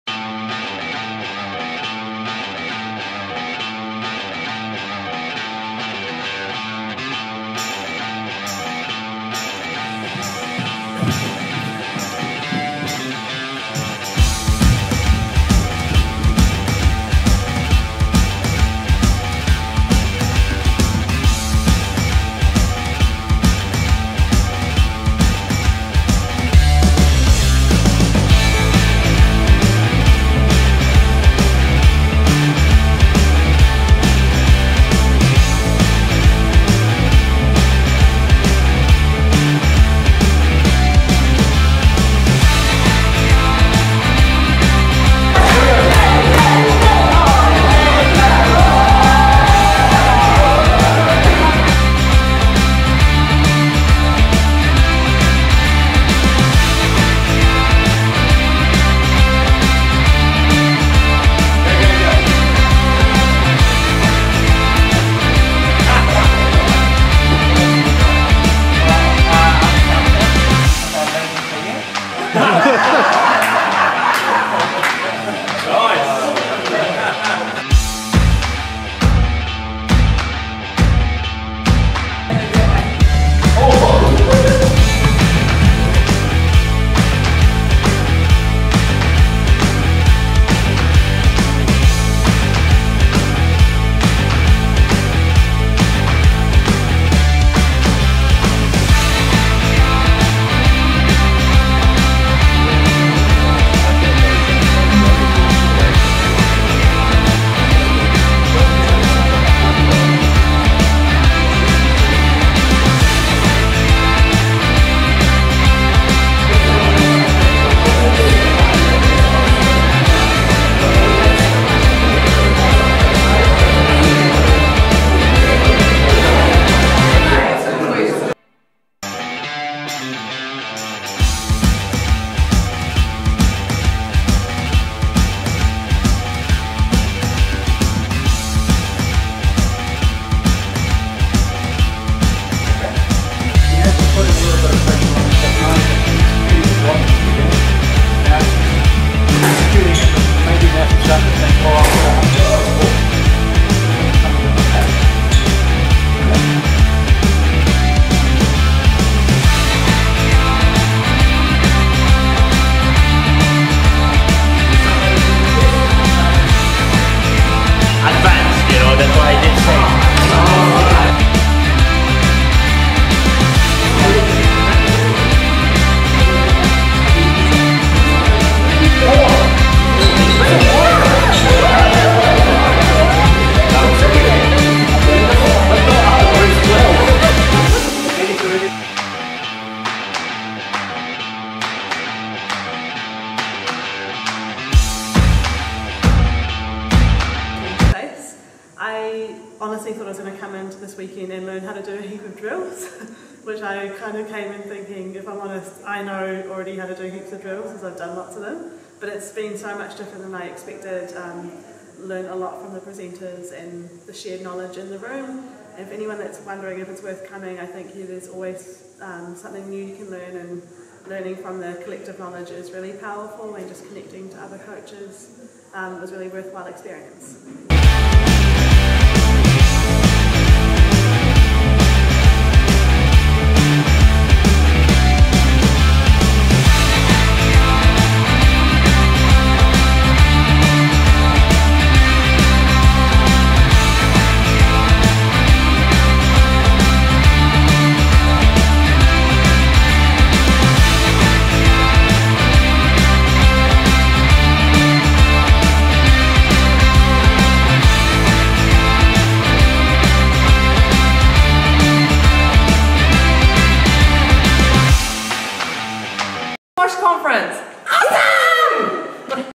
Squash NZ National Coaching Conference - March 23
Recap of the National Coaching Conference held at the National Squash Centre in Auckland from 3-5 March 2023